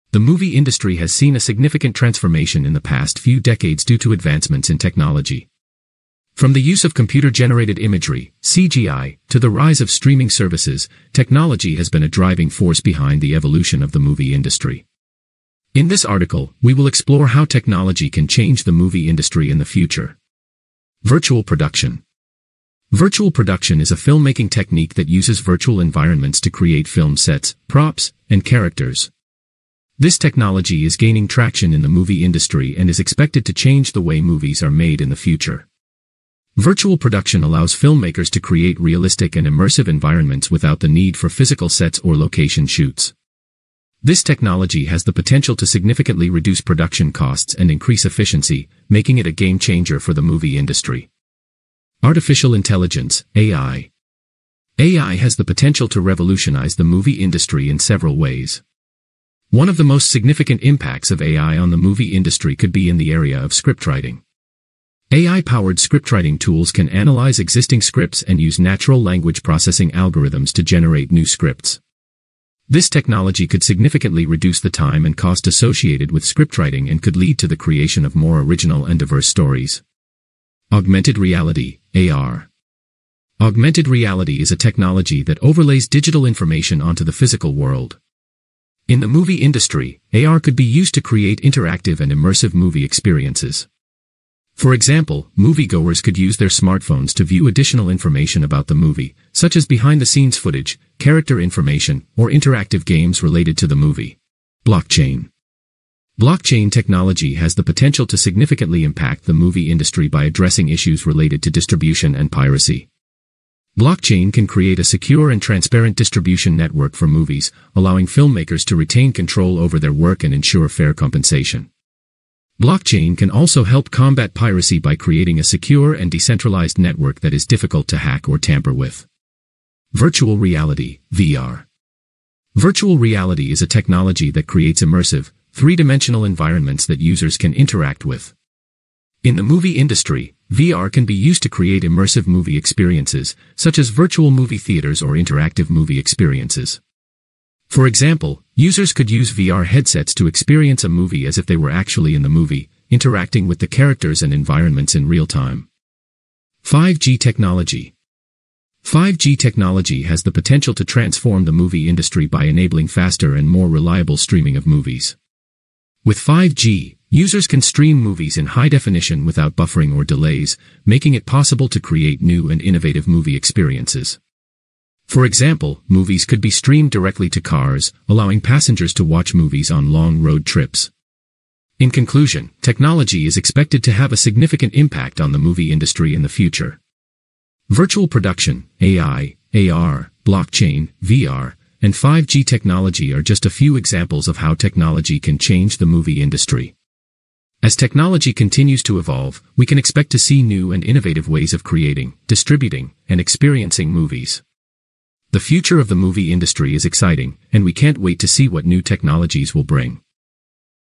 How Technology Can Change The Movie Industry In The Future
The movie industry has seen a significant transformation in the past few decades due to advancements in technology. From the use of computer-generated imagery (CGI) to the rise of streaming services, technology has been a driving force behind the evolution of the movie industry. In this article, we will explore how technology can change the movie industry in the future.

Virtual Production
Virtual production is a filmmaking technique that uses virtual environments to create film sets, props, and characters. This technology is gaining traction in the movie industry and is expected to change the way movies are made in the future. Virtual production allows filmmakers to create realistic and immersive environments without the need for physical sets or location shoots. This technology has the potential to significantly reduce production costs and increase efficiency, making it a game-changer for the movie industry.

AI has the potential to revolutionize the movie industry in several ways. One of the most significant impacts of AI on the movie industry could be in the area of scriptwriting. AI-powered scriptwriting tools can analyze existing scripts and use natural language processing algorithms to generate new scripts. This technology could significantly reduce the time and cost associated with scriptwriting and could lead to the creation of more original and diverse stories.

Augmented Reality (AR)
Augmented reality is a technology that overlays digital information onto the physical world. In the movie industry, AR could be used to create interactive and immersive movie experiences. For example, moviegoers could use their smartphones to view additional information about the movie, such as behind-the-scenes footage, character information, or interactive games related to the movie.

Blockchain
Blockchain technology has the potential to significantly impact the movie industry by addressing issues related to distribution and piracy. Blockchain can create a secure and transparent distribution network for movies, allowing filmmakers to retain control over their work and ensure fair compensation. Blockchain can also help combat piracy by creating a secure and decentralized network that is difficult to hack or tamper with.

Virtual reality is a technology that creates immersive, three-dimensional environments that users can interact with. In the movie industry, VR can be used to create immersive movie experiences, such as virtual movie theaters or interactive movie experiences. For example, users could use VR headsets to experience a movie as if they were actually in the movie, interacting with the characters and environments in real-time.

5G Technology
5G technology has the potential to transform the movie industry by enabling faster and more reliable streaming of movies. With 5G, users can stream movies in high-definition without buffering or delays, making it possible to create new and innovative movie experiences. For example, movies could be streamed directly to cars, allowing passengers to watch movies on long road trips.

In conclusion, technology is expected to have a significant impact on the movie industry in the future. Virtual production, AI, AR, blockchain, VR, and 5G technology are just a few examples of how technology can change the movie industry. As technology continues to evolve, we can expect to see new and innovative ways of creating, distributing, and experiencing movies. The future of the movie industry is exciting, and we can't wait to see what new technologies will bring.